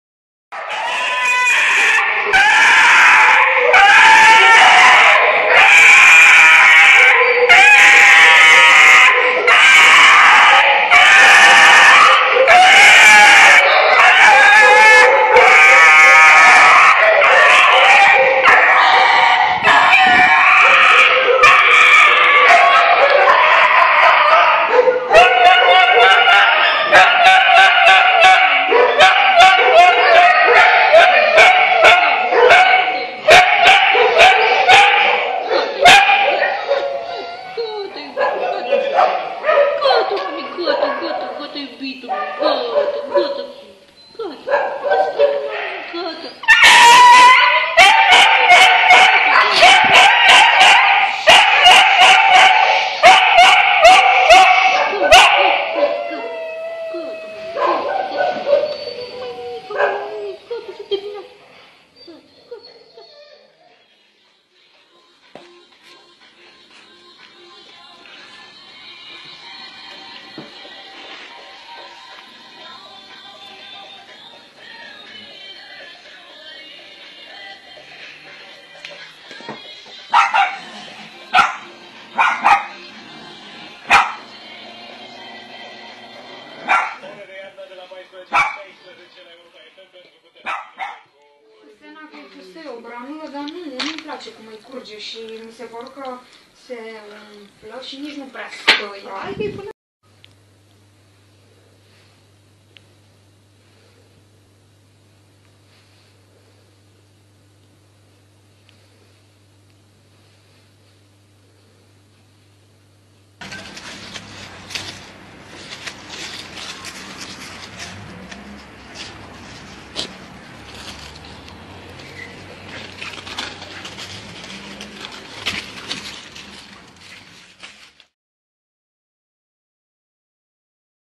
Sen se stal skutečností...
Velmi silné video, jehož první část v listopadu obletěla svět. Prosím vydržte prvních pár desítek sekund, kde je vidět pejsek Kayne v rumunském útulku, kde se ho jeho zachránkyně snaží pohladit. Pejsek musel být nepopsatelně týrán, protože jeho naříkání a strach rve člověku srdce. Byl však zachráněn a rodina z Anglie, která video uviděla se okamžitě rozhodla, že ho musí adoptovat, aby tento pejsek již nikdy v životě nazažil podobné utrpení. Jeho proměna vás zahřeje u srdce a snad ty méně soucitné alespoň osloví, aby se třeba o tuto problematiku začali zajímat a možná časem i pomáhat... aby třeba i ti, co si zvířata kupují, si je začali adoptovat… Tam venku jich jsou miliony a trpí jen kvůli nám… Všechny příběhy se šťastným koncem, by ho štastný nikdy neměly, pokud by se někde, někdo nerozhodl, že jim pomůže… Buďte jejich anděly… Díky vám všem, kteří jimi už jste…